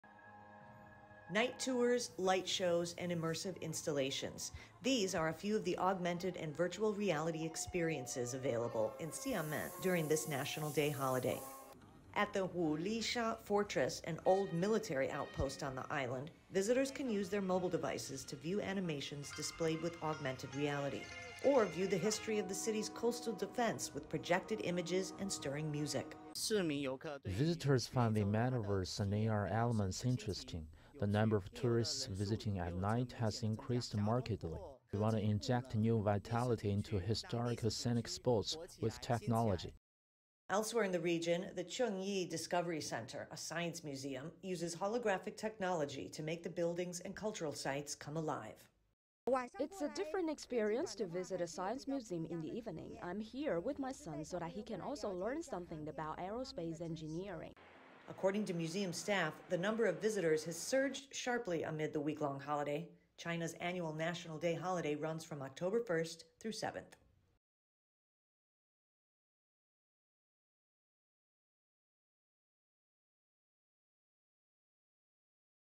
Immersive virtual experience provides new attraction for Chinese holiday makers
Augmented and virtual reality experiences are available in many tourist attraction spots across China during the last National Day holiday, as well as night tours.